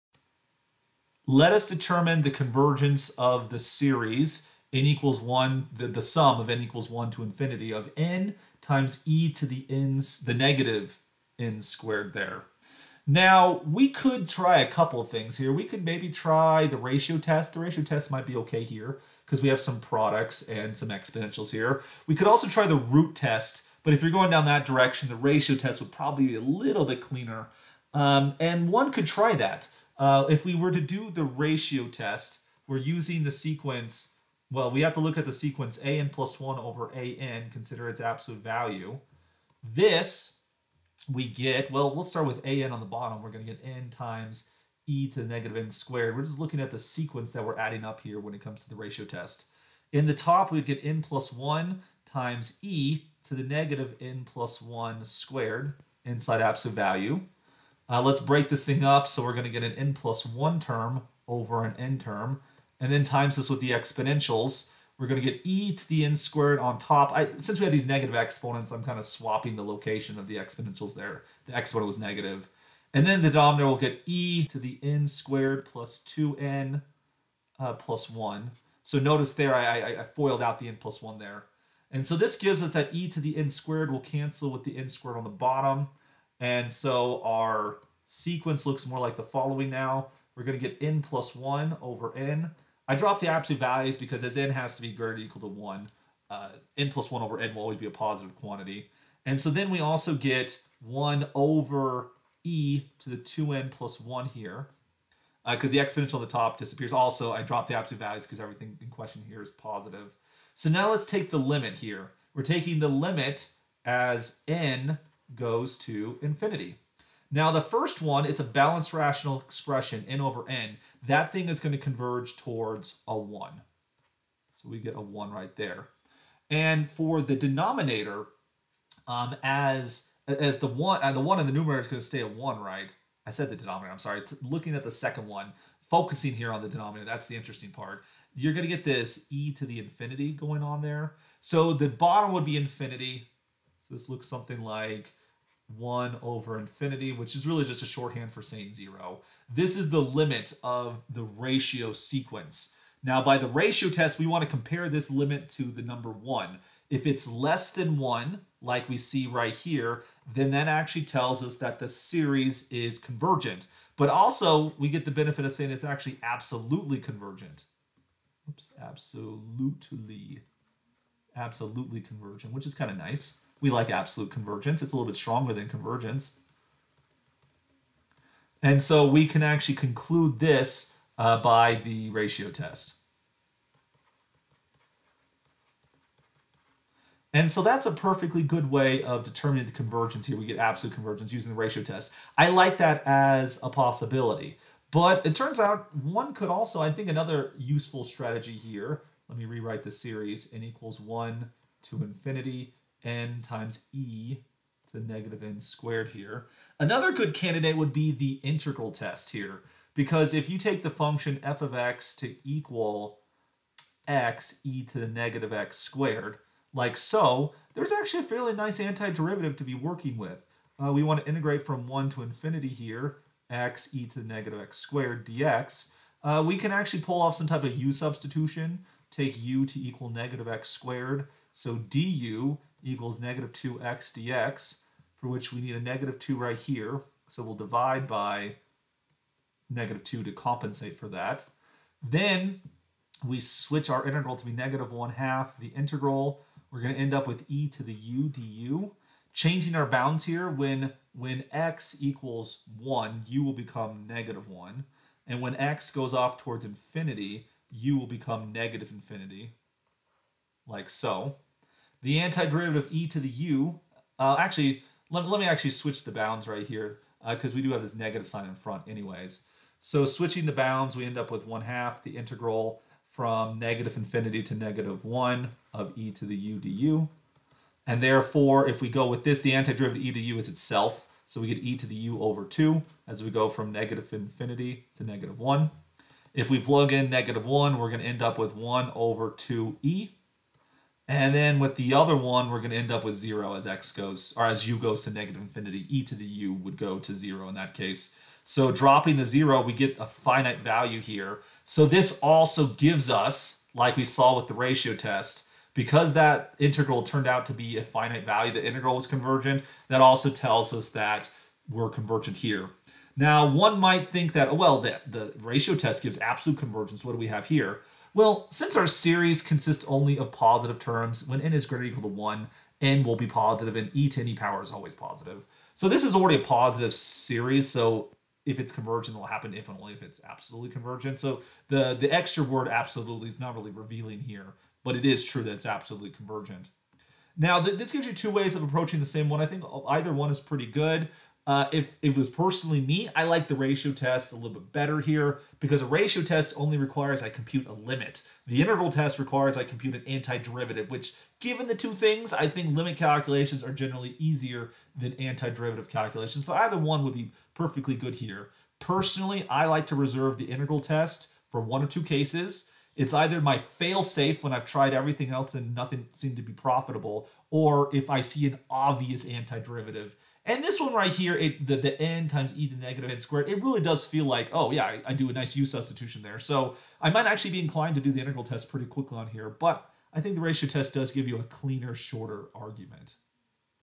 Street Fighting Infinite Series - Part V
We present here a series of videos about determining the convergence of series using various convergence test.

This is lecture 42 (part 6/10) of the lecture series offered by Dr. Andrew Misseldine for the course Math 1220 - Calculus II at Southern Utah University. A transcript of this lecture can be found at Dr. Misseldine's website or through his Google Drive